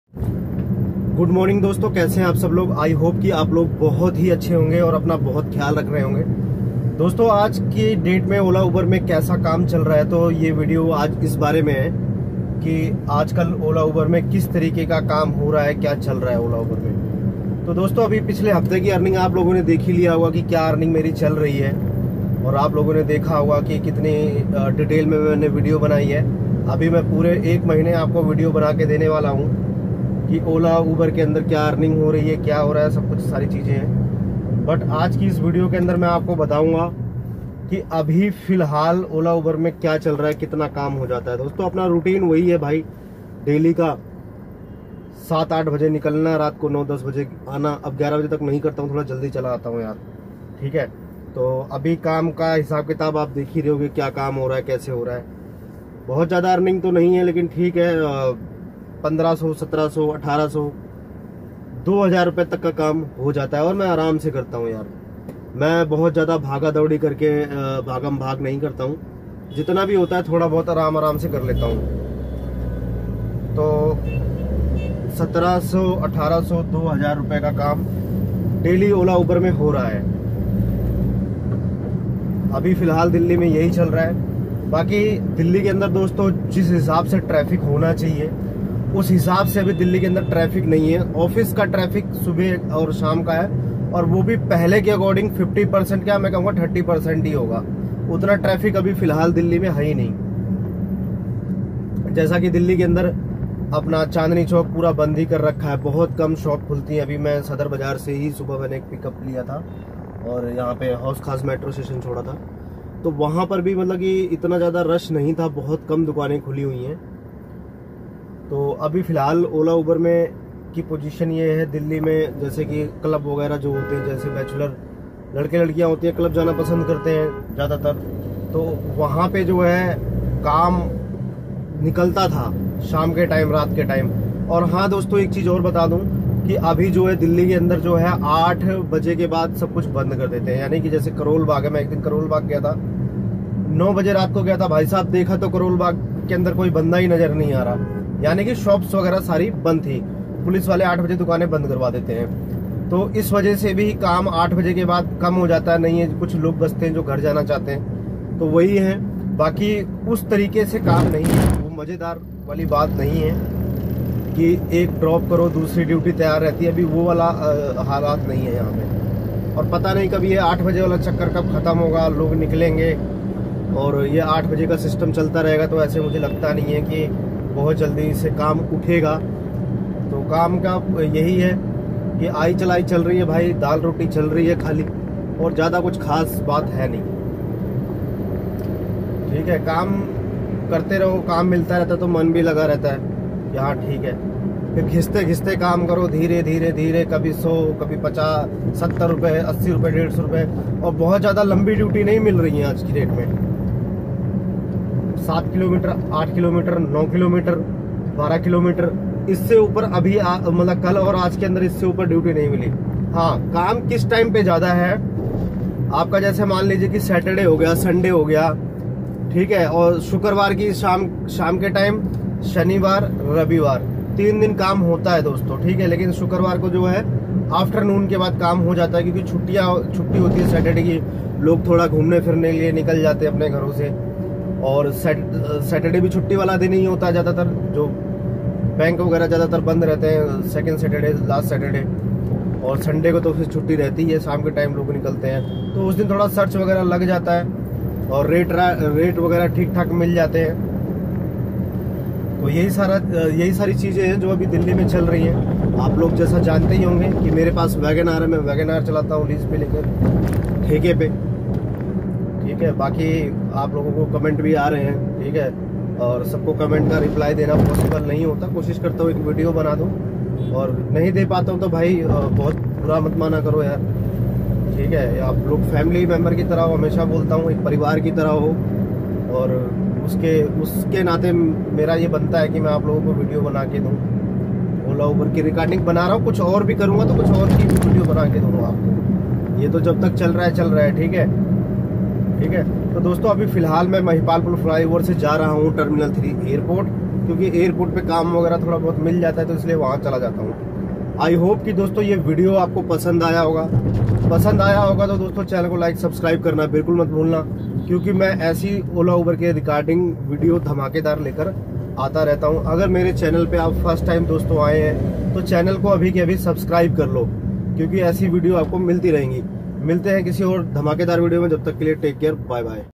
What is Going On in Ola Uber Right Now, how Much is Earning in Delhi-NCR
What is Going On in Ola Uber Right Now, how Much is Earning in Delhi-NCR ‎|| Pukhraj K Videos